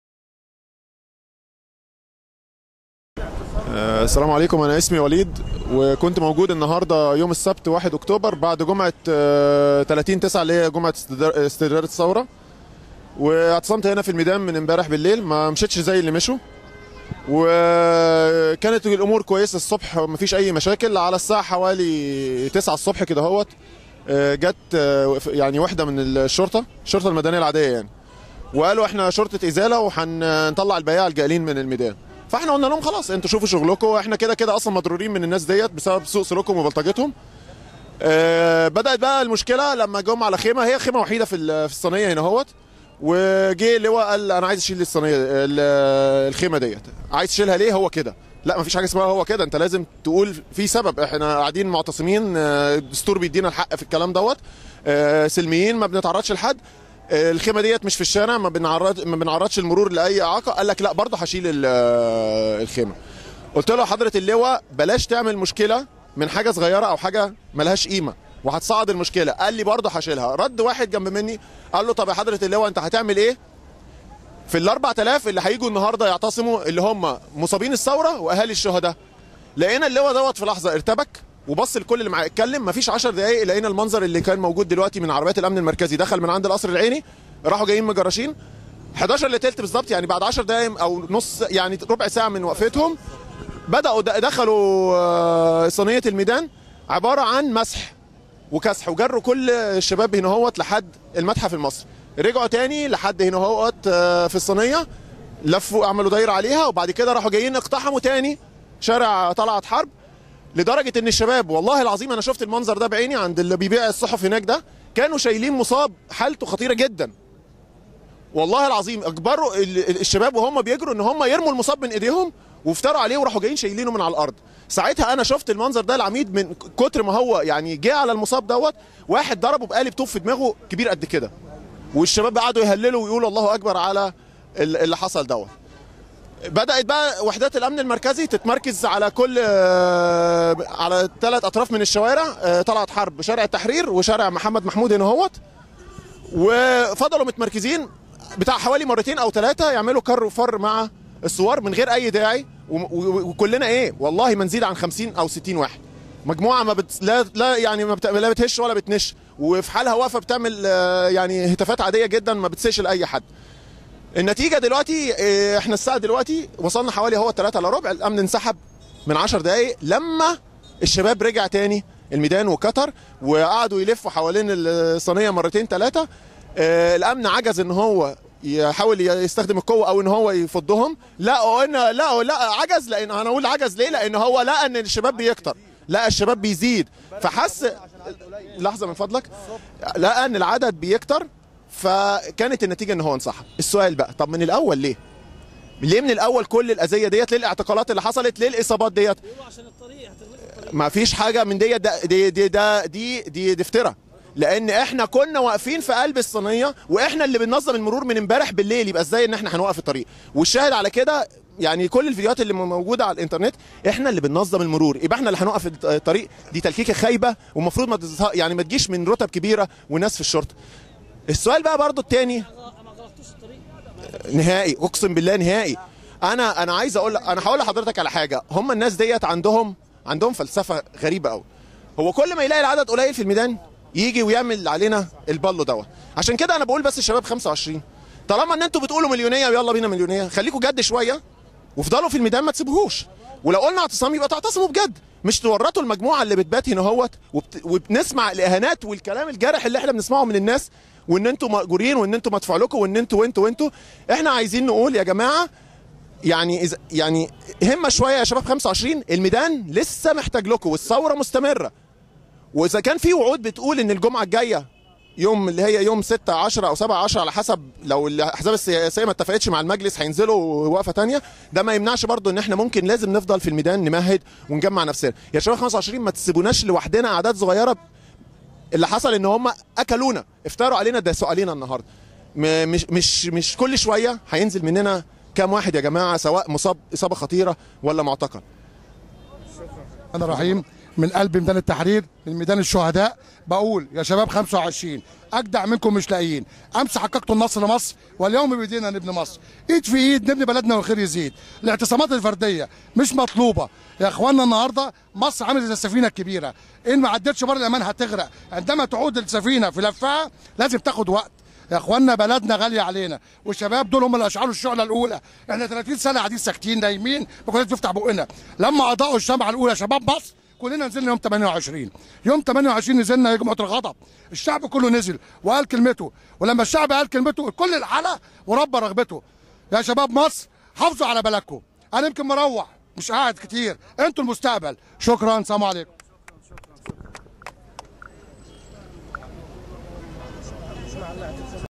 شاهد عيان على وقعة إشتباكات ميدان التحرير صباح السبت
شاهد عيان على وقعة إشتباكات ميدان التحرير صباح السبت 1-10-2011